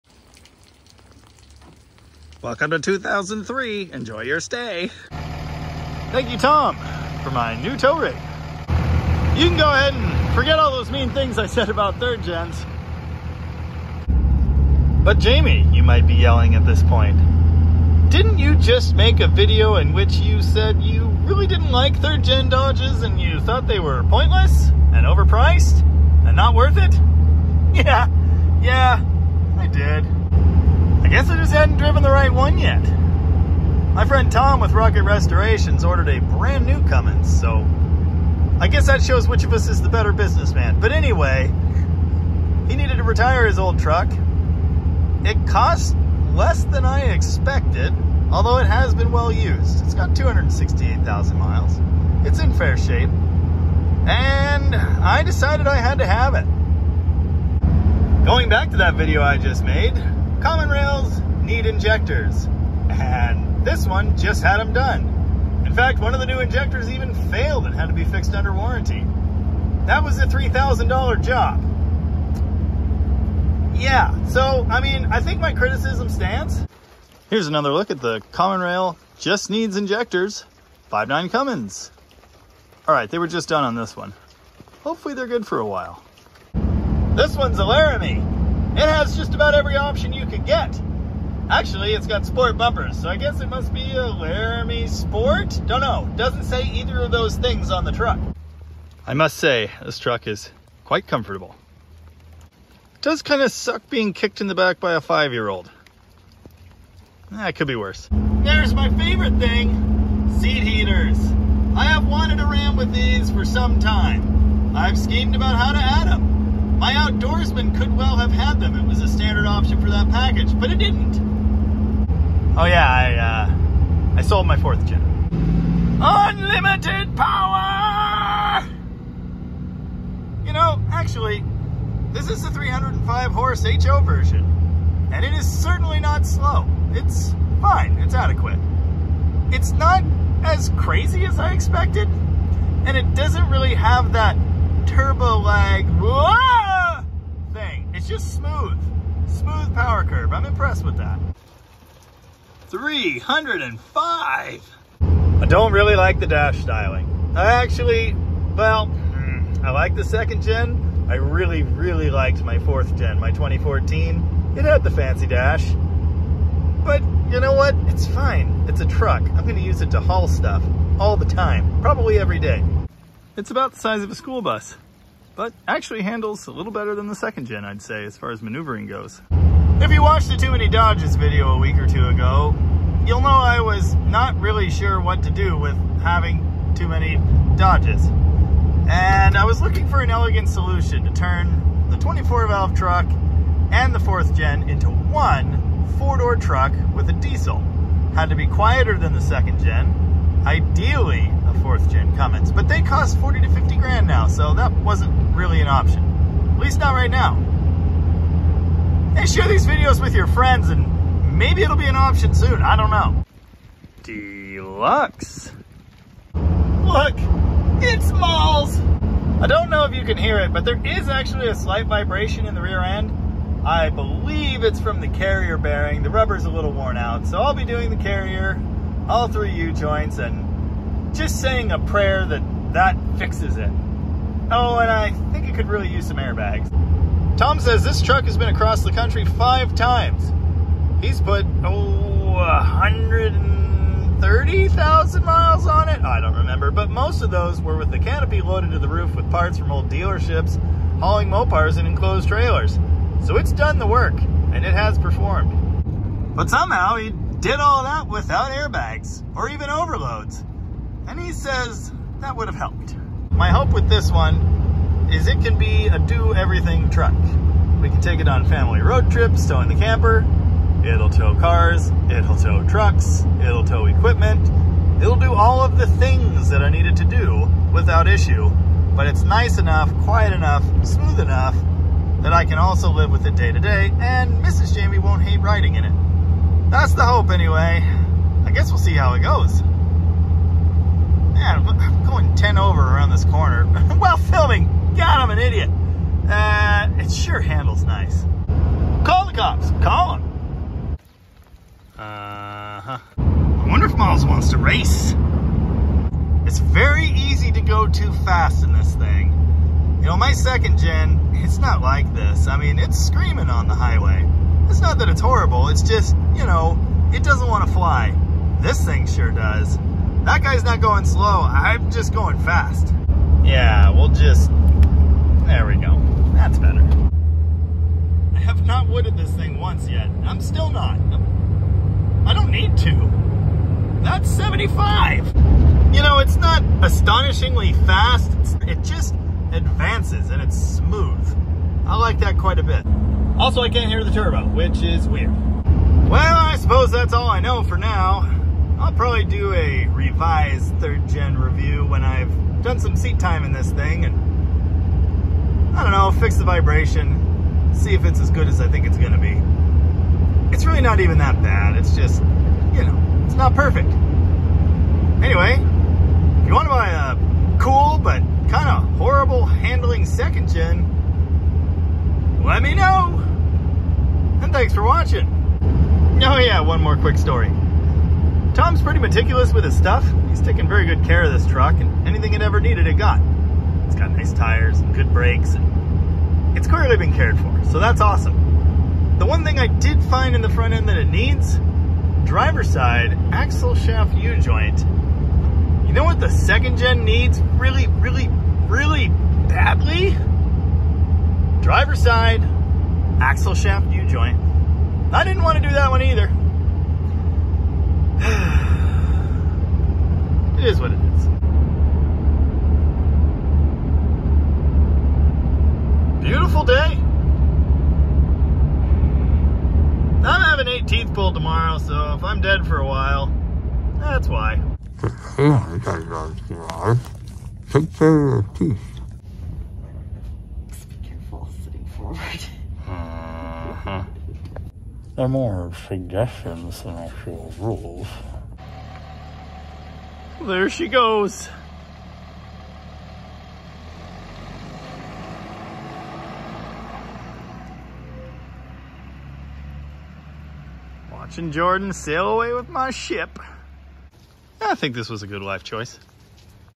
OK, I Was Wrong - The Third Gen Cummins Is Pretty Good - Dead Dodge Garage
Last week I made a video criticizing these trucks for being more expensive than second gens, not having enough room inside compared to second and fourth gens, and often needing fuel injectors replaced. And this week, I went ahead and bought one anyway. While a lot of my criticism stands… this is a great driving truck, and it’s much, much quieter and nicer to ride in than any of the second gens I’ve had. I’m happy with my purchase - although I did sell my fourth gen V6 truck (which I loved) to make it happen.